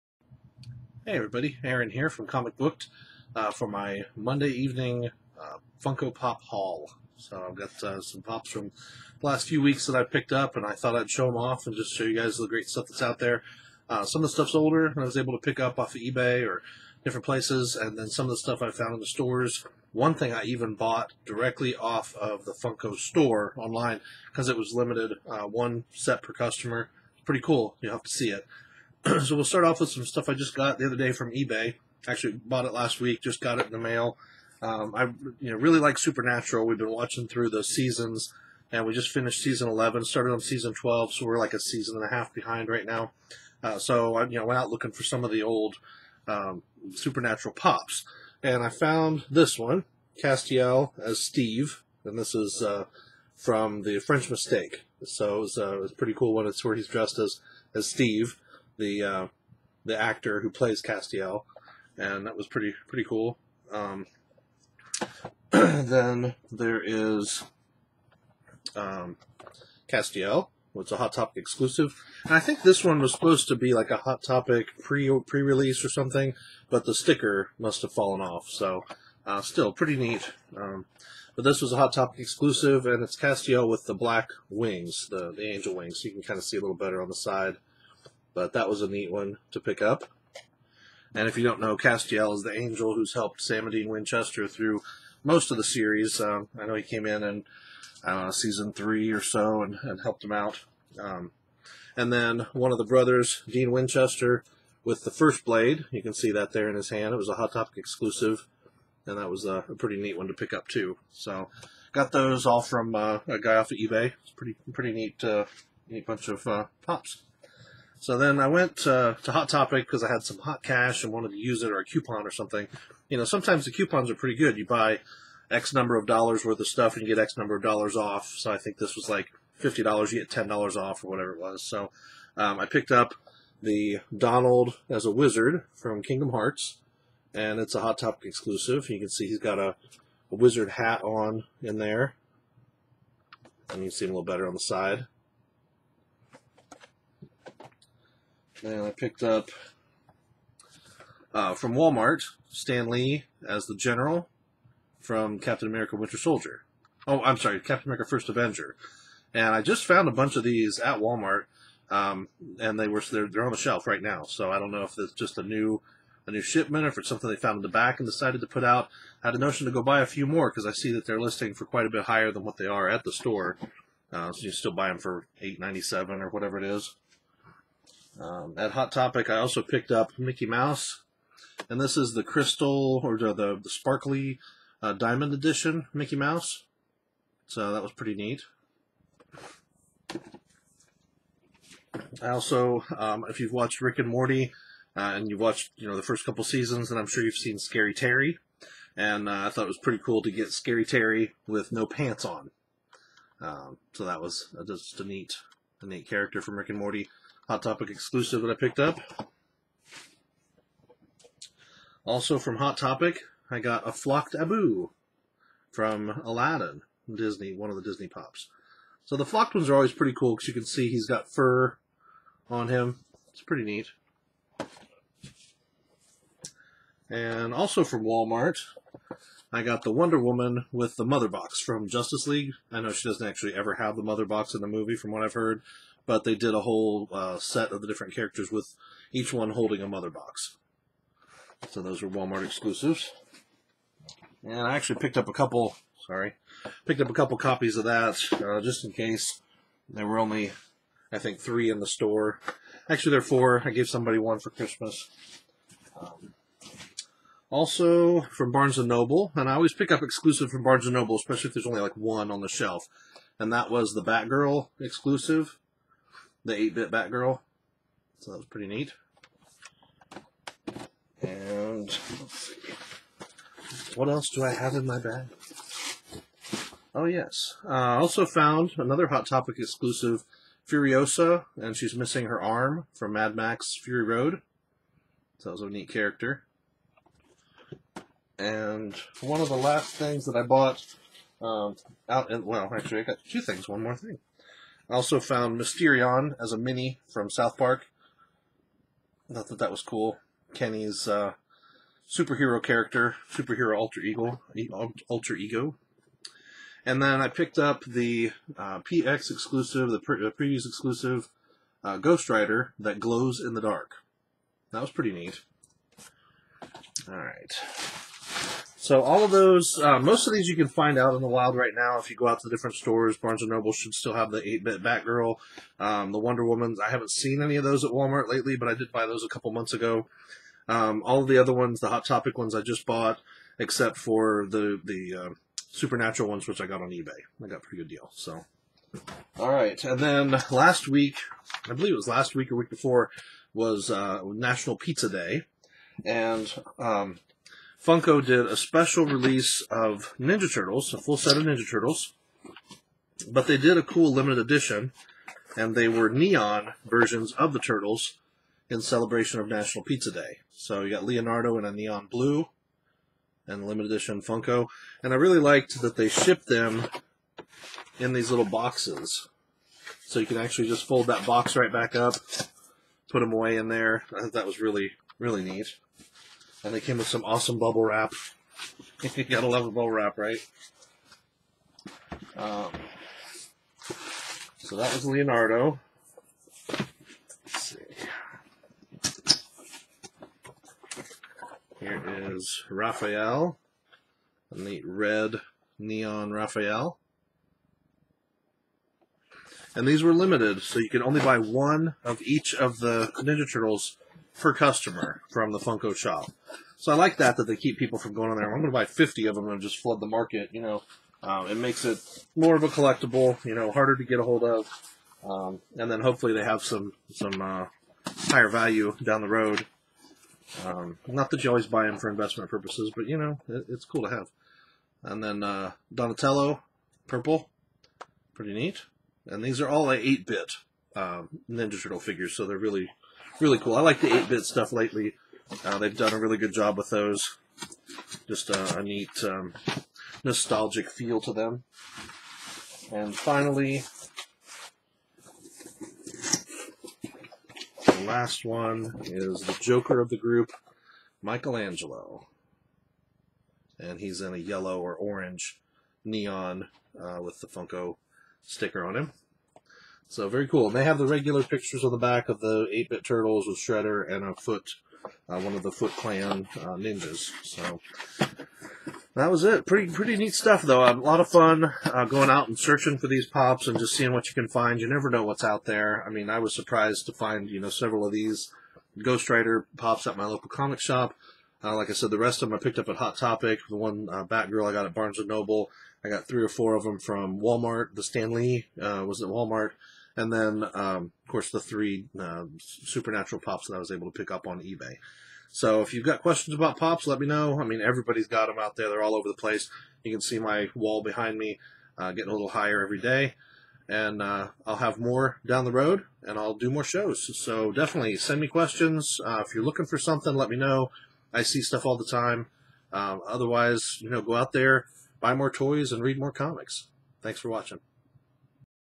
Comic Booked Pop Hunt - February 2018
Always out there looking for some cool Funko Pop figures, here is the next installment of the Comic Booked Pop Hunt for February 2018.